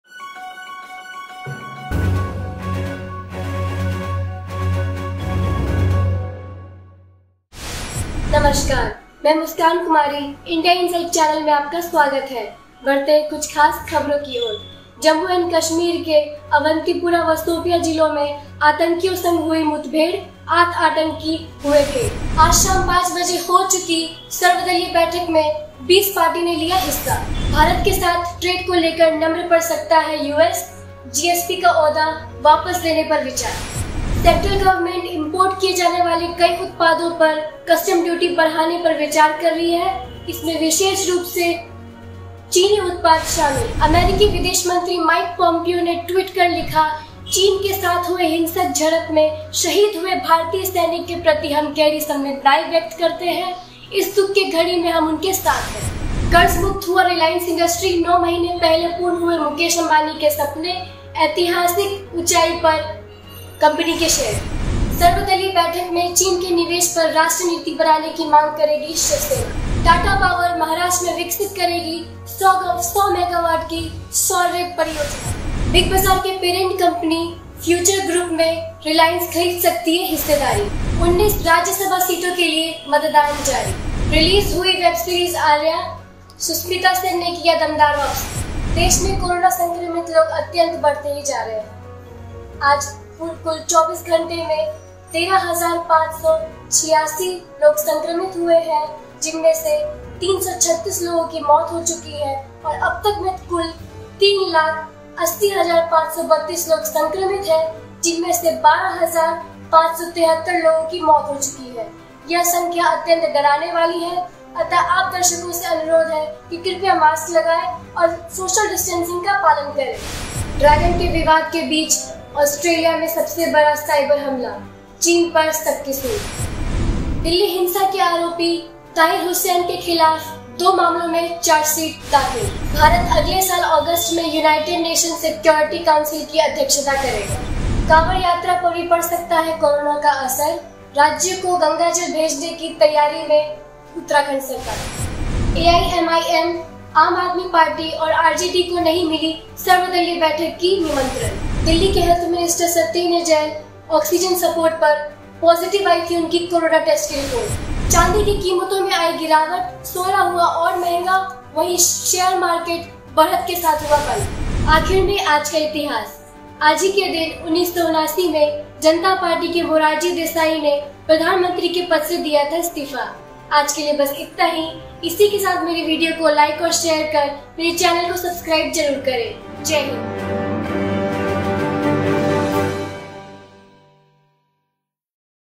DAILY NEWS #05 Top news in 3min COVID-19||AllPartyMeet||GSP||UNSC||CustomDuty||Sensex||IND0-CHINA
Featuring - Muskan Kumari
Edited By - Sourav Kumar

The India Insight के साथ देखिये देश विदेश से जुड़े सभी महत्वपूर्ण खबरें|